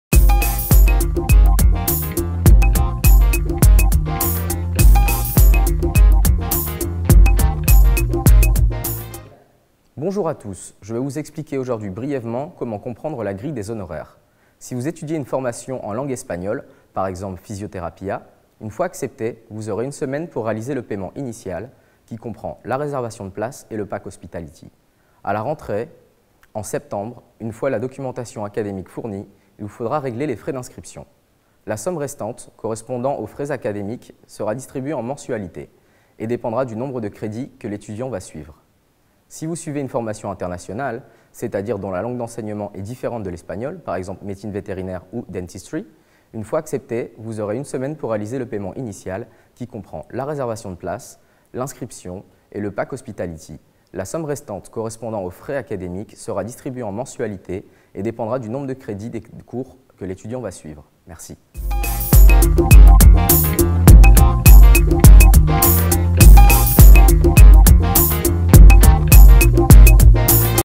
Explication des honoraires des formations à l'Université CEU Cardenal Herrera
Dans cette vidéo, nous vous expliquons plus sur notre grille des honoraires.